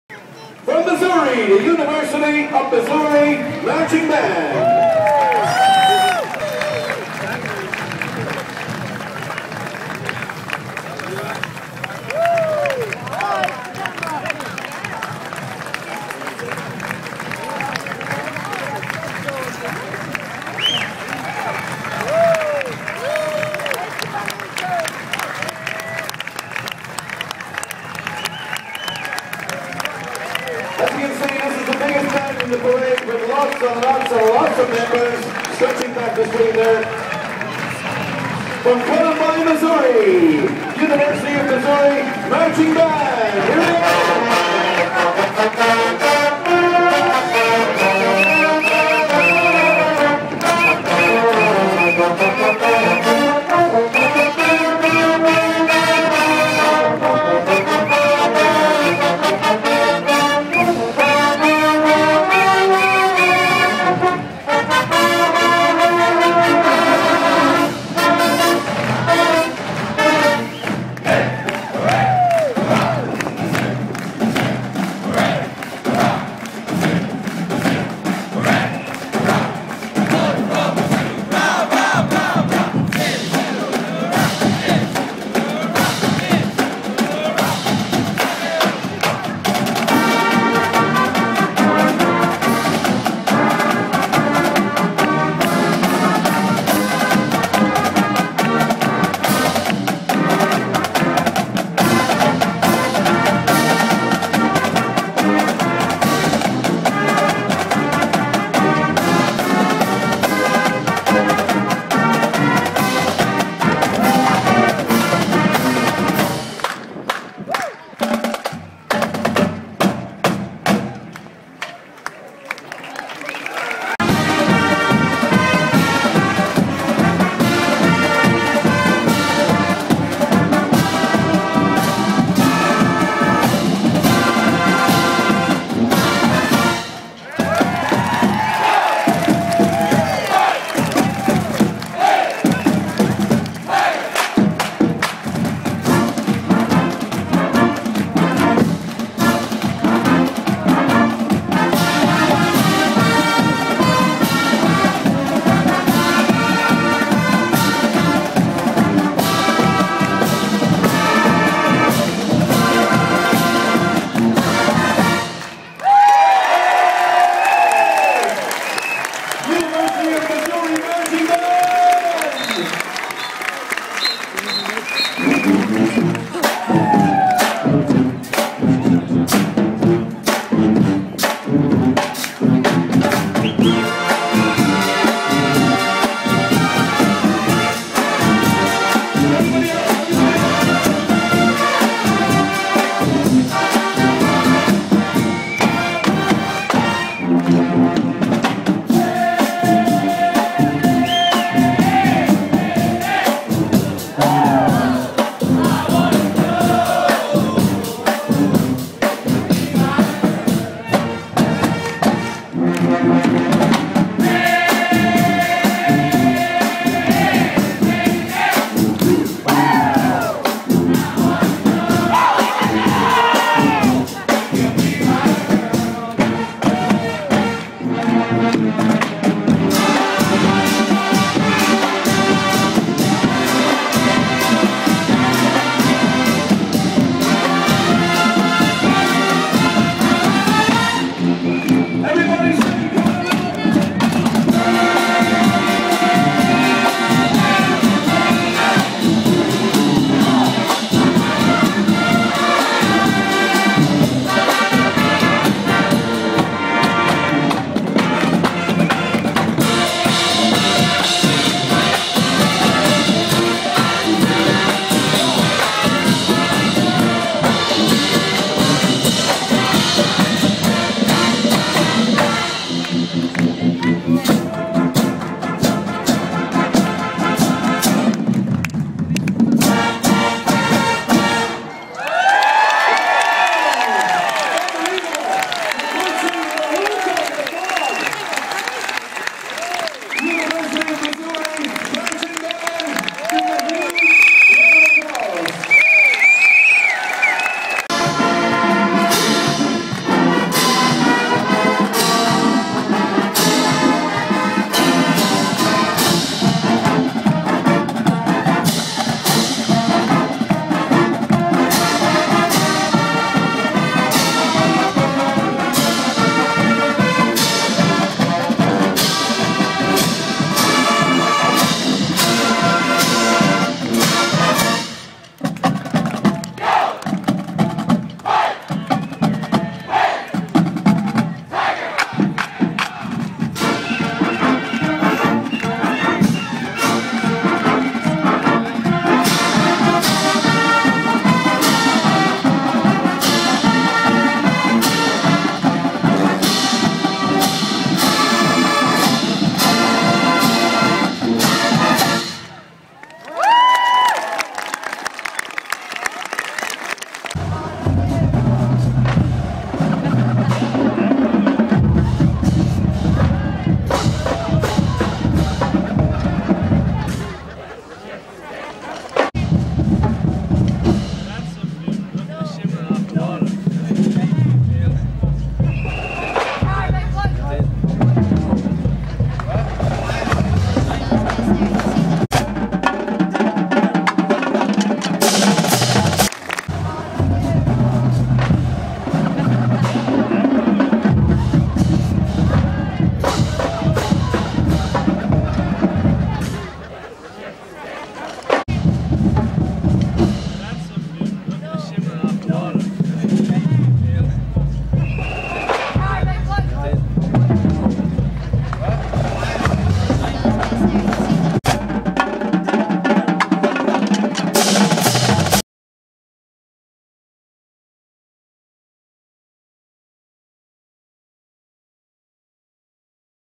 Marching Mizzou in Limerick ,St.Patrick's Weekend 2012.
UNIVERSITY OF MISSOURI, MARCHING MIZZOU, LIMERICK  IRELAND
2012 for the Limerick International Band Parade 2012.